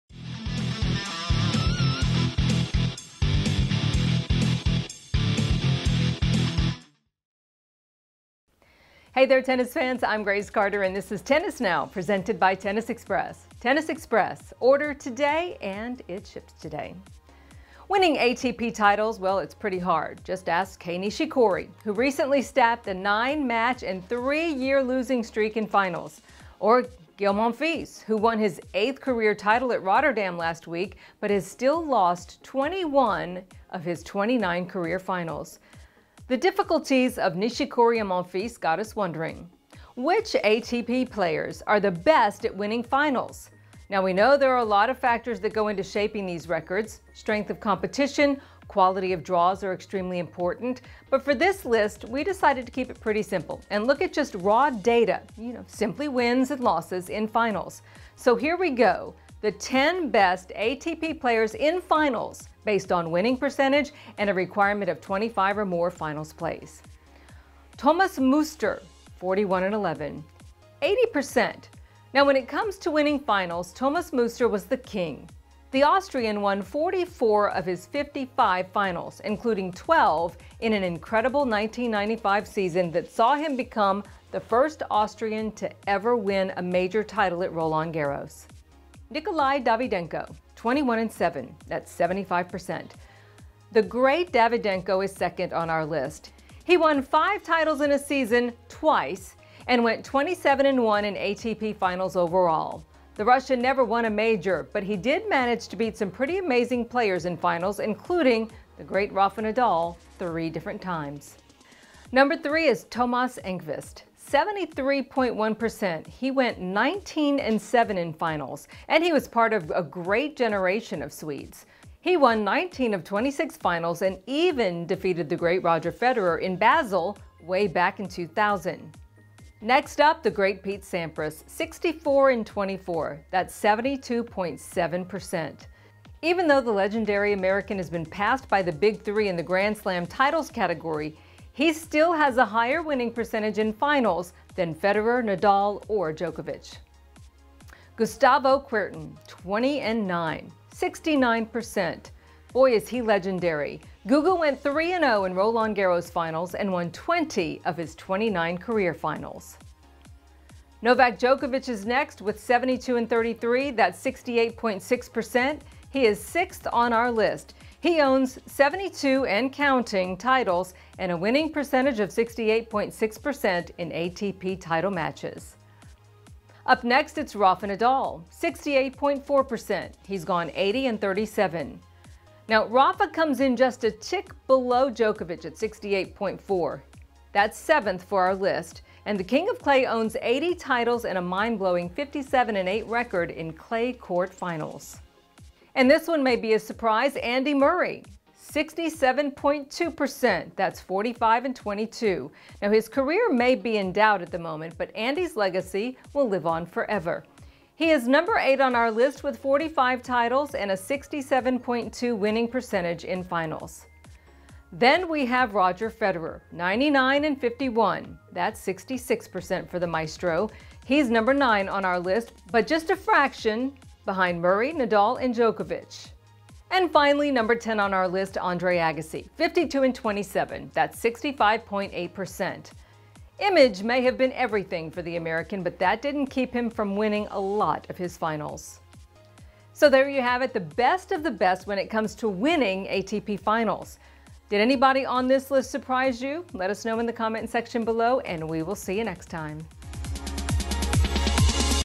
Captains of Clutch: Which ATP Players are Best in Finals?
Which ATP players were the best at winning finals? That’s what we’ve been wondering at Tennis Now this week, so we figured we’d dig up the numbers and bring them to you. Without any further ado, here are the ATP’s 10 best player in finals, by winning percentage.